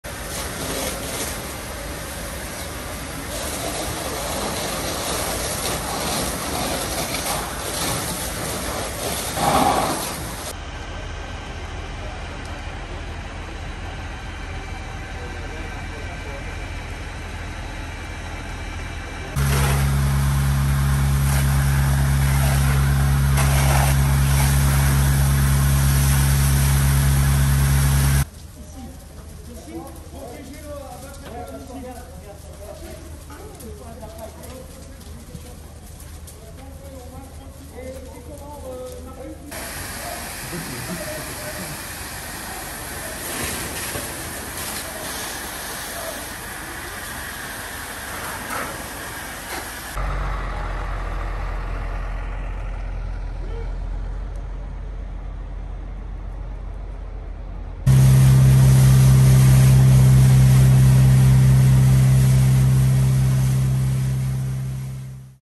SDIS02 Feu de bâtiment agricole - OrignyEnThiérache
Les sapeurs-pompiers de l'Aisne se sont mobilisés pour lutter contre un important feu, impactant un bâtiment agricole, dans la nuit du 10 au 11 octobre 2021. Une forte synergie des risques a rendu l'opération particulièrement intense : mais l'intervention des sapeurs-pompiers a permis d'éviter une propagation à un hangar attenant. Pas de victime.